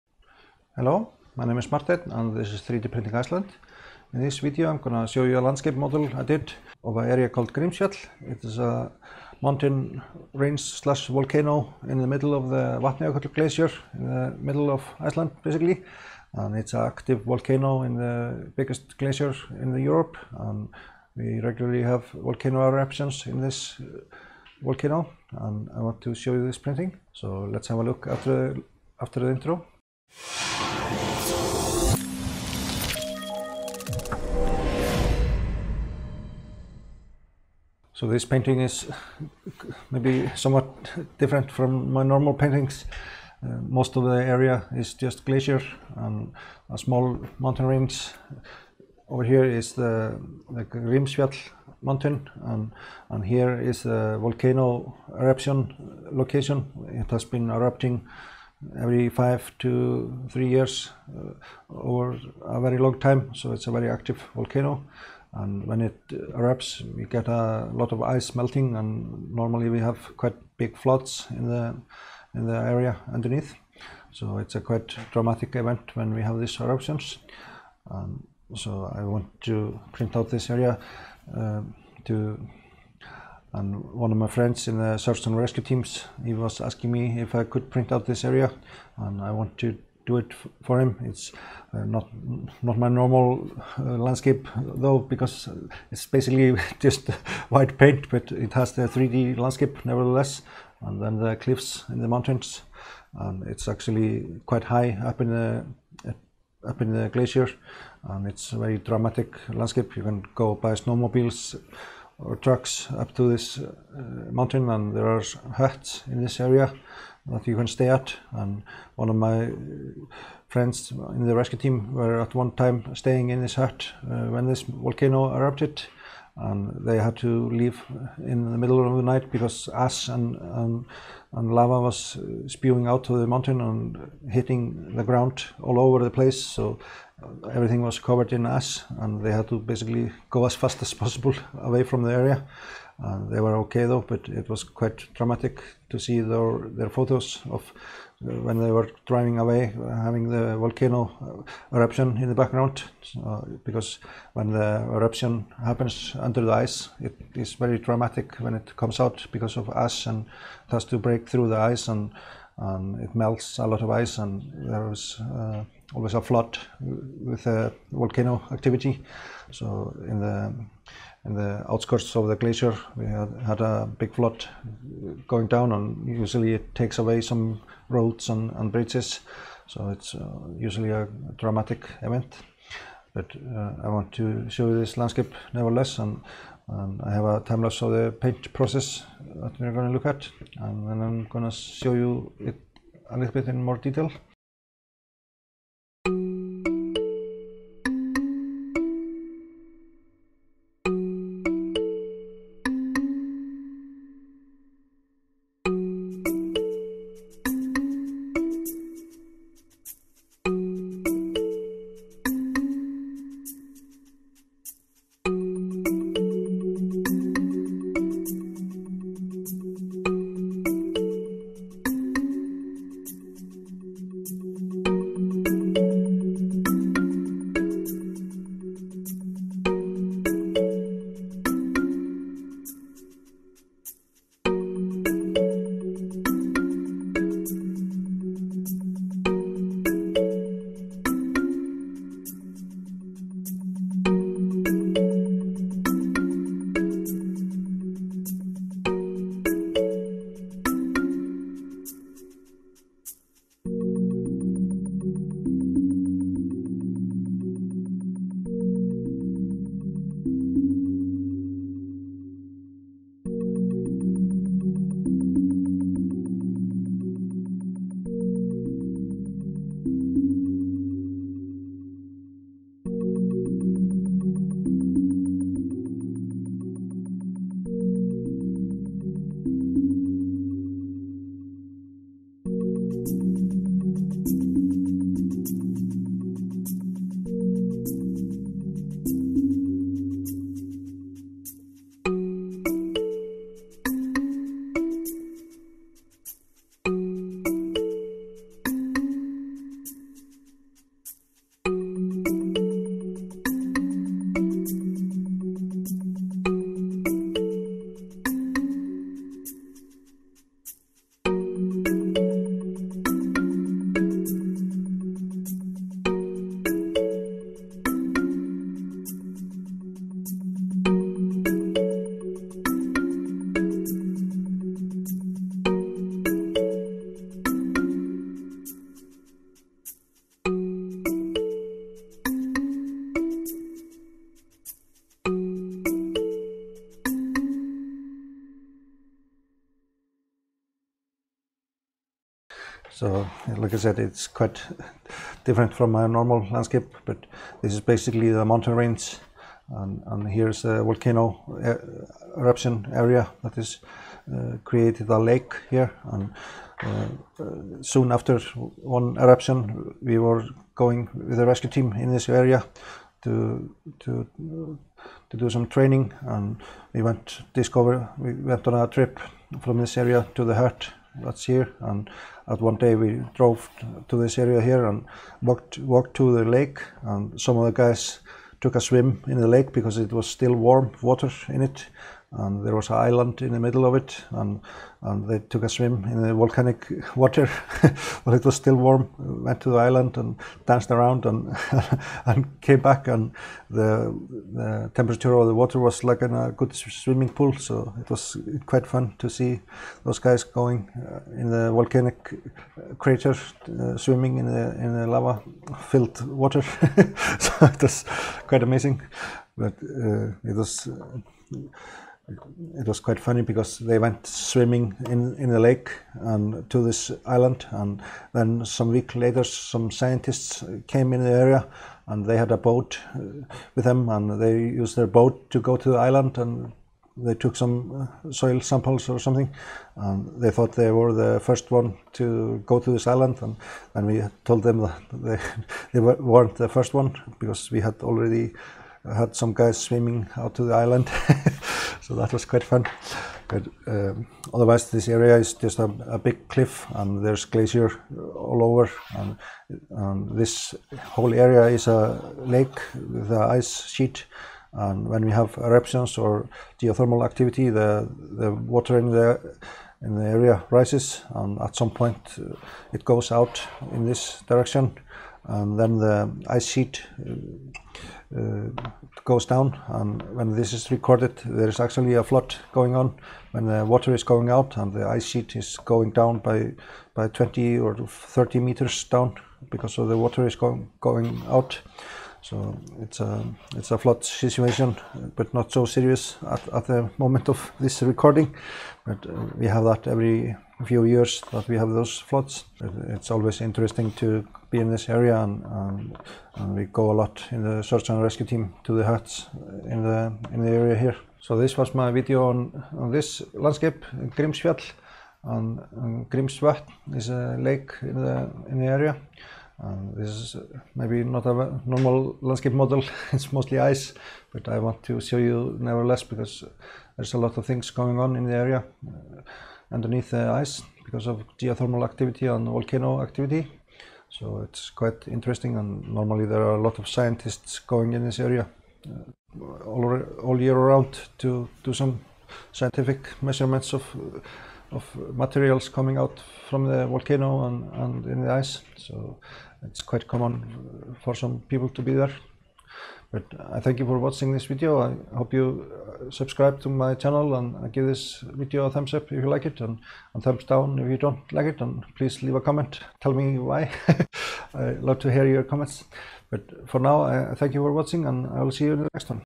3D Printed and Painted Landscape Grimsfjall
In this video I print out and painta a 3D Model of Grimsfjall, a volcano that is in the middle of Vatnajökull glacier.